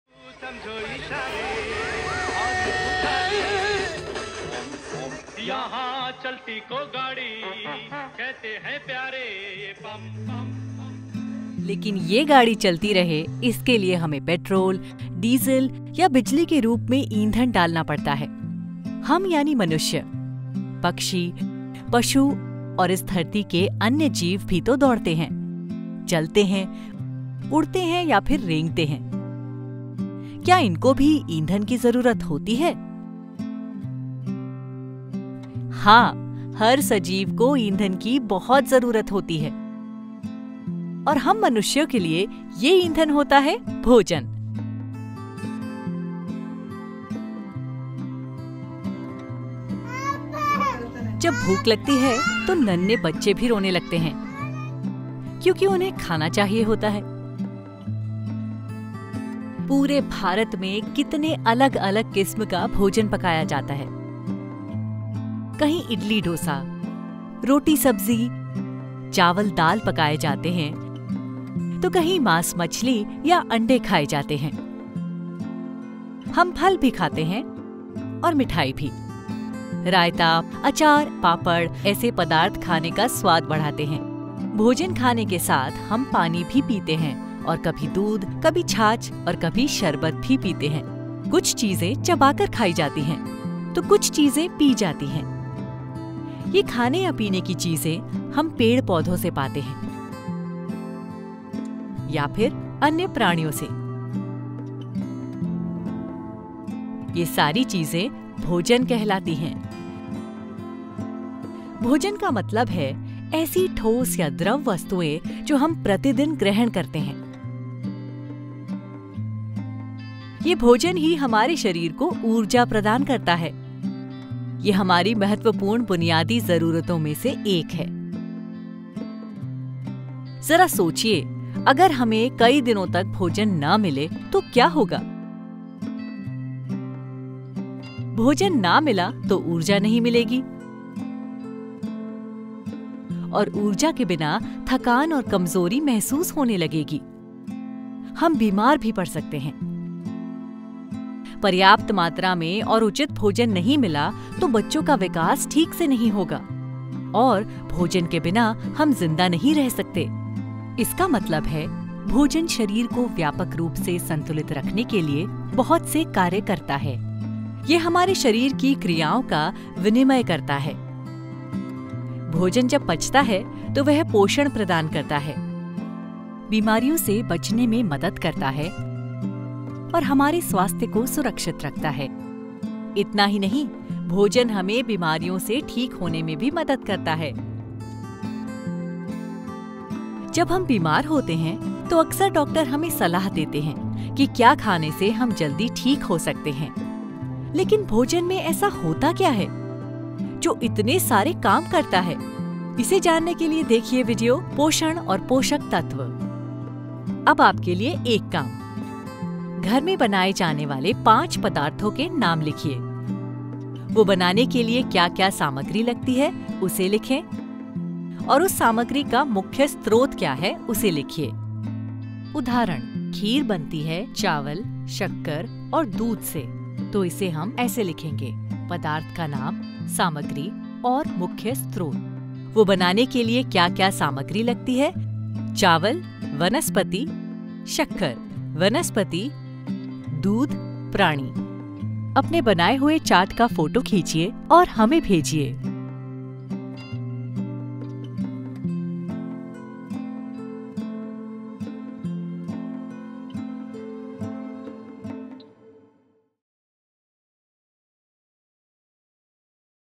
Food: One of our basic needs (Hindi) | भोजन : हमारी बुनियादी जरूरतों में से एक
Discover why food is essential for us and how it provides energy. Just like all living beings, humans need food to function. This video explores the diversity of Indian cuisine and the nutritional importance of food.